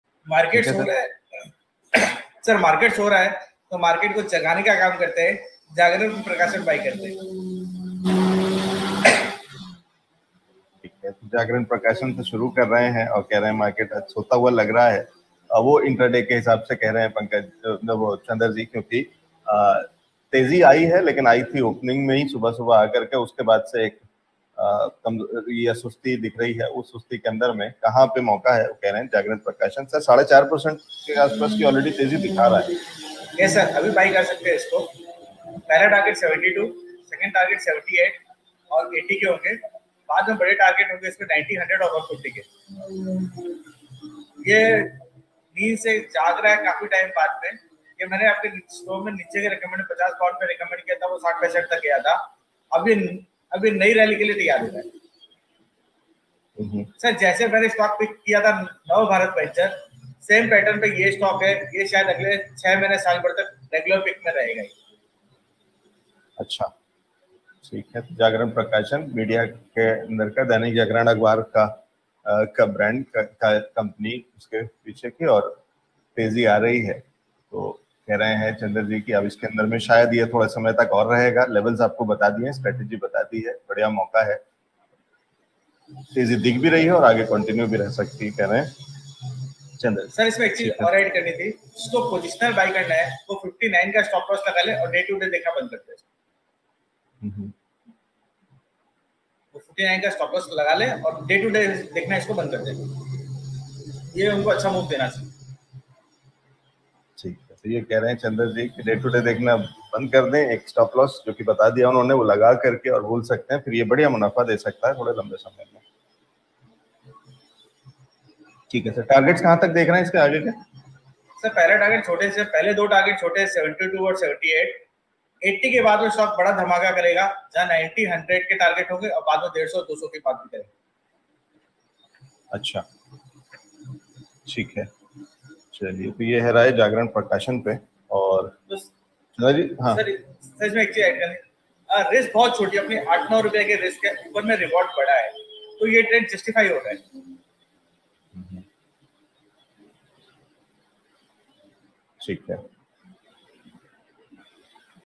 Jagran Prakashan Ltd. के शेयर में क्या करें? Expert Recommendation by Chander Surana
जानिए हमारे Stock Market Expert Chander Surana की राय |

Analysis of: Jagran Prakashan Ltd.

Shows on StockMarket equity trading investment commodity personalfinance tax cryptocurrency blockchain reviews jagranprakashan marketanalysis startups policyreviews nifty sensex and much more.